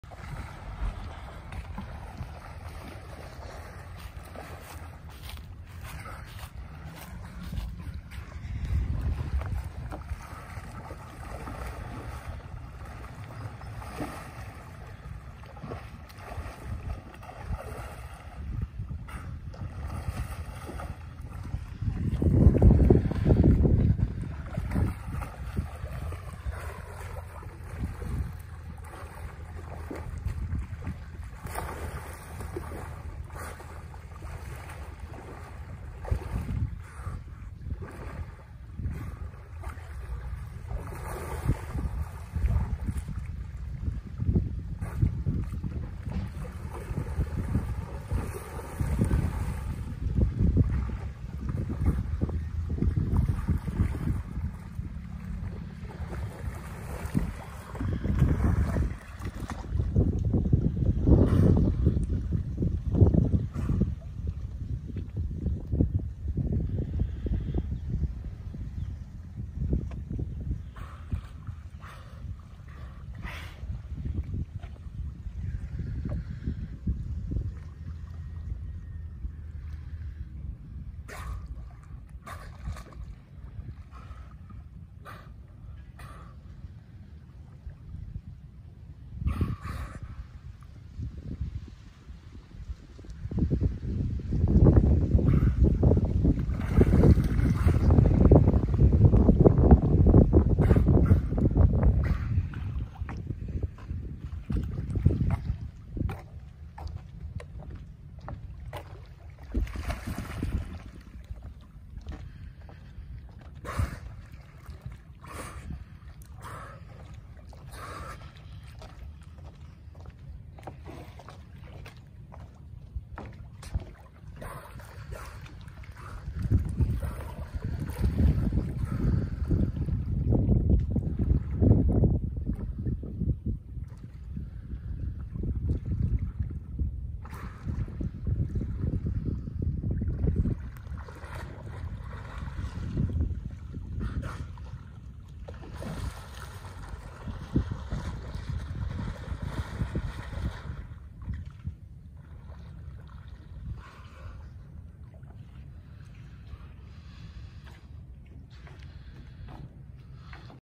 Beagle Point dolphins Jan 2021
Beagle Point dolphins take action on a winter morning